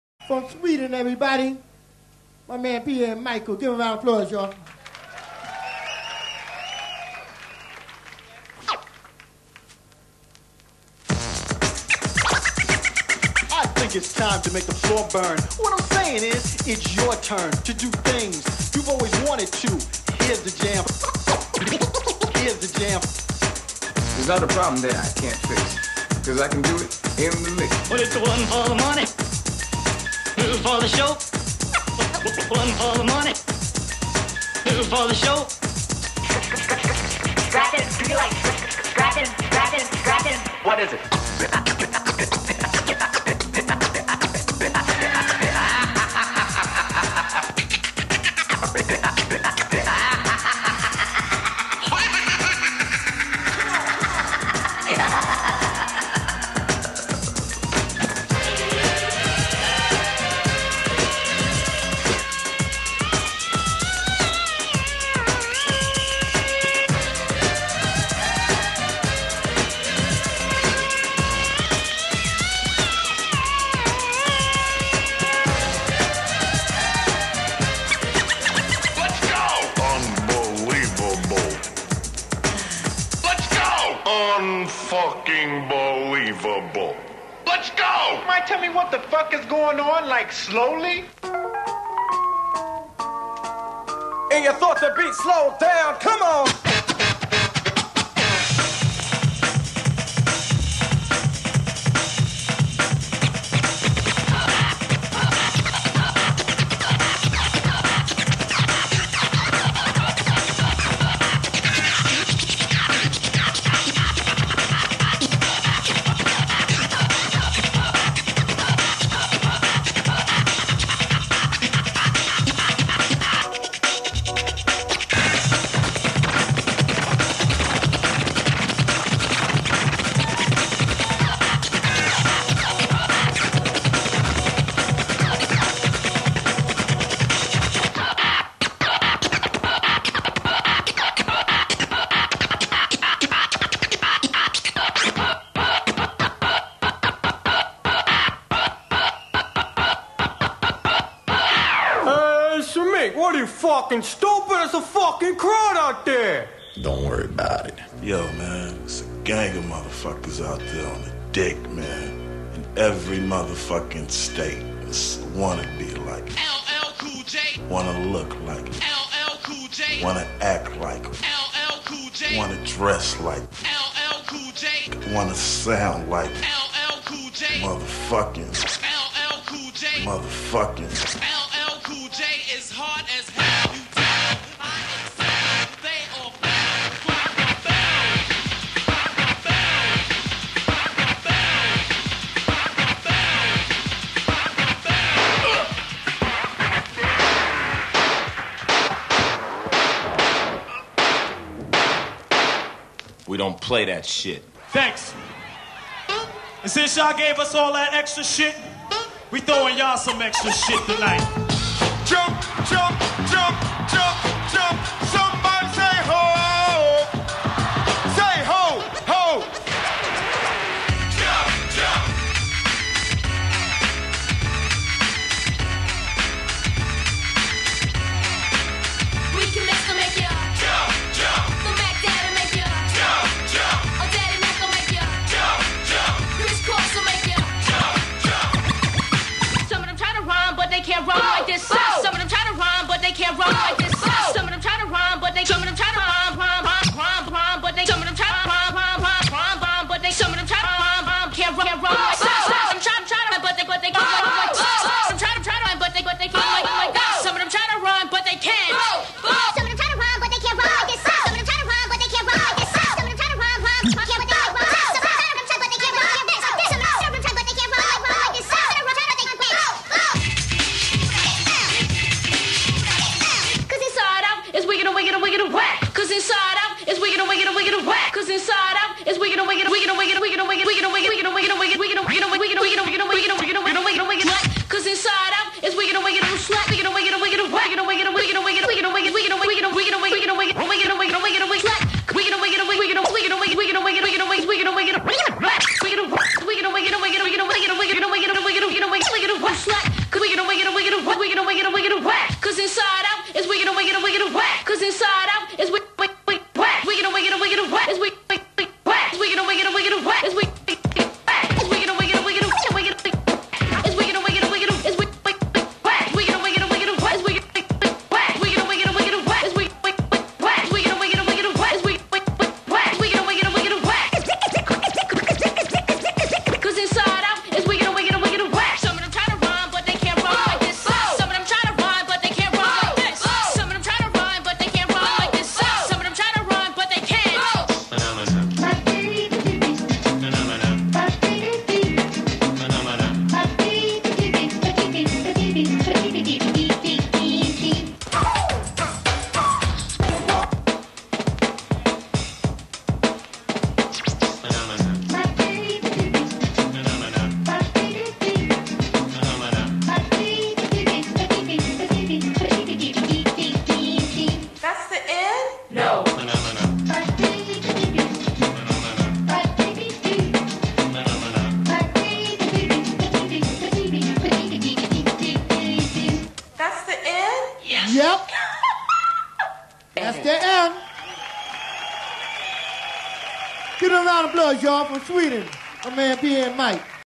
DMC World 1992 Pierre Jerksten (Suécia)
Boa Performance da dupla...interessante eles "matando" o LL Cool J..."confira"